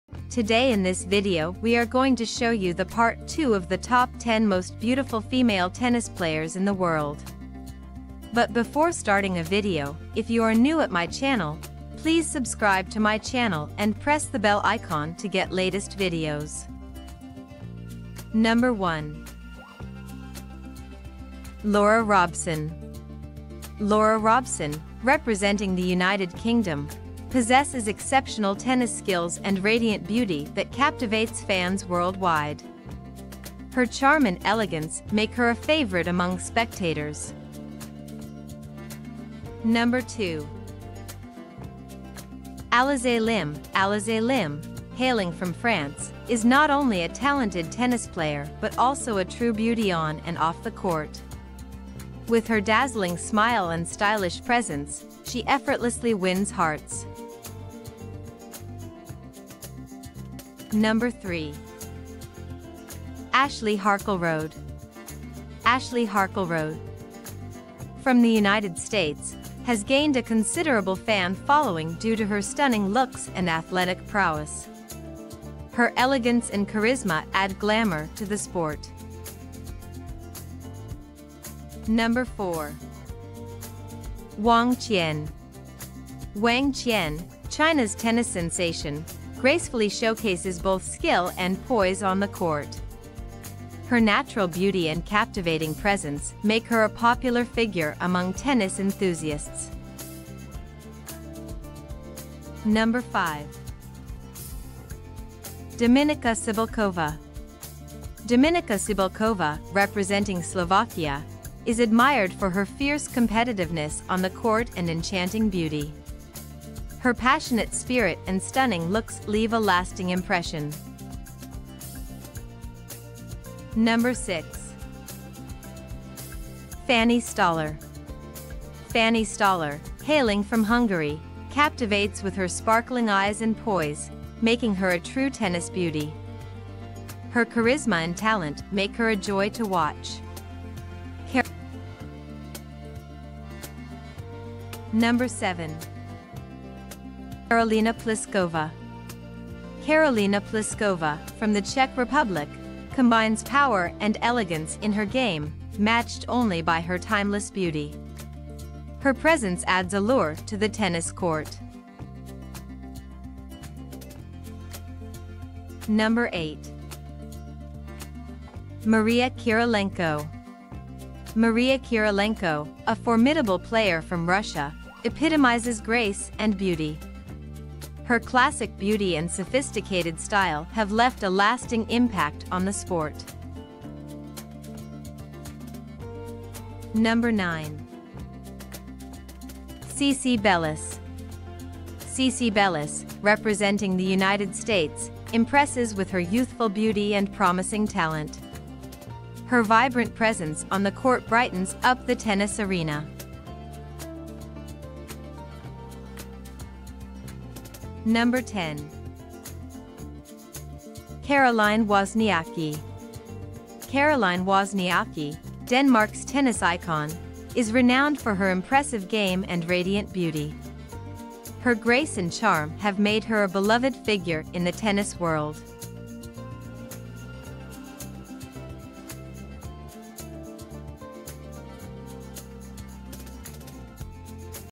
Top 10 Most Beautiful Female Tennis Players In The World || Beautiful Tennis Players ( Part 2 )
Today in this video we are going to show you the part 2 of the most beautiful female tennis players in the world. This is the part 2 of the most beautiful female tennis players in the world.